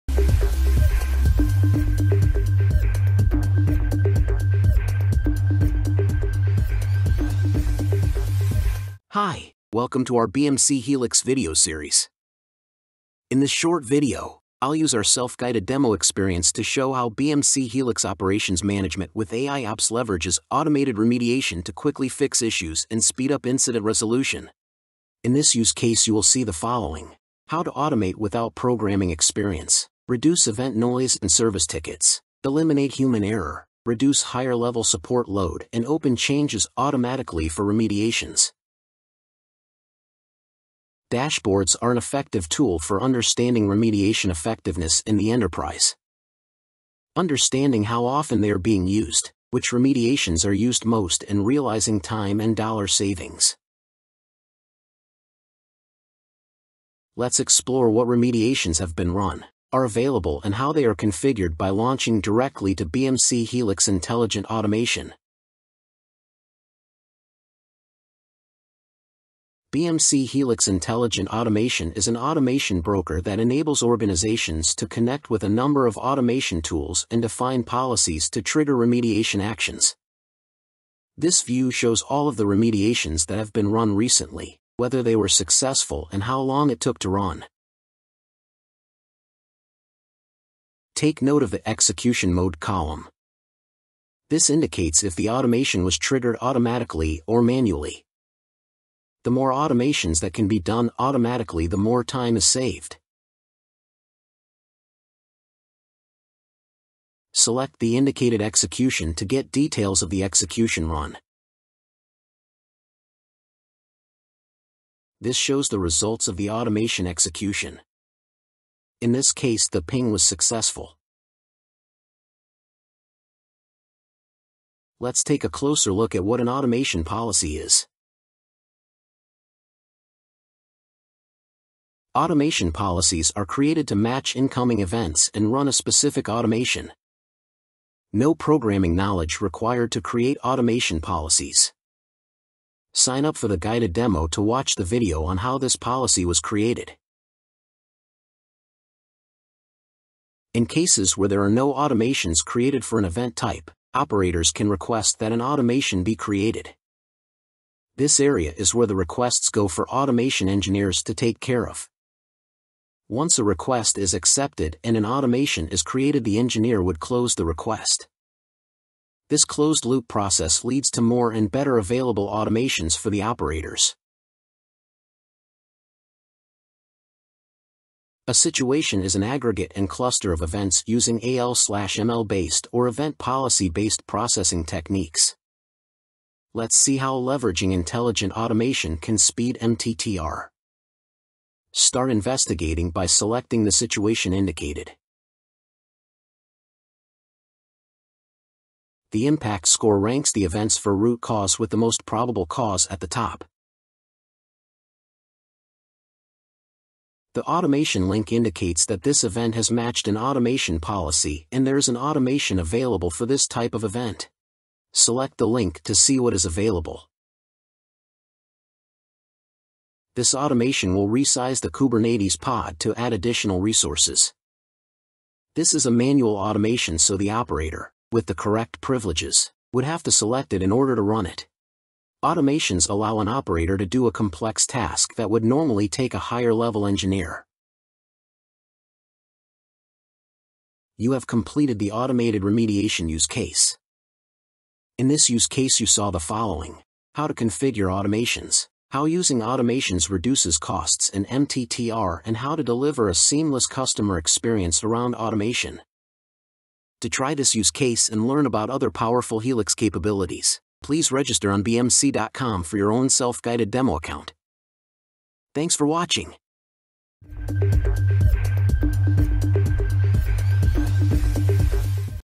Quickly Fix Recurring Problems Automatically Saving Time and Money with Automated Remediation
Quickly fix recurring problems automatically saving time and money with BMC Helix Operations Management with AIOps and Automated Remediation.